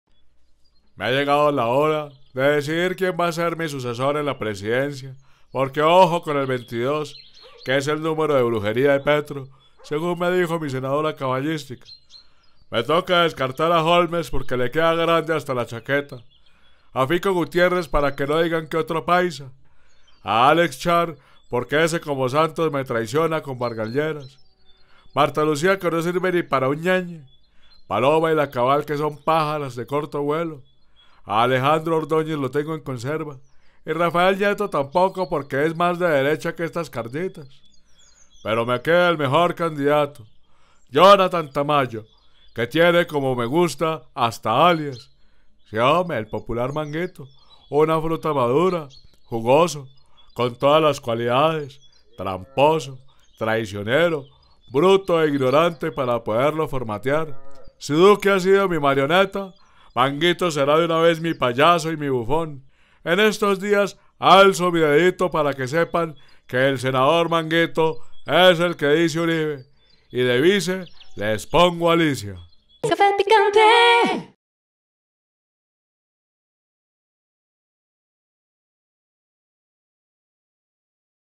URIBE CON SU DEDITO DESIGNA SU CANDIDATO PARA 2022: EL SENADOR MANGUITO
APÓYANOS

Donaciones Bitcoin: 16Vwsi8jSLZ791cPXQeuKbSmiirR9n5QuS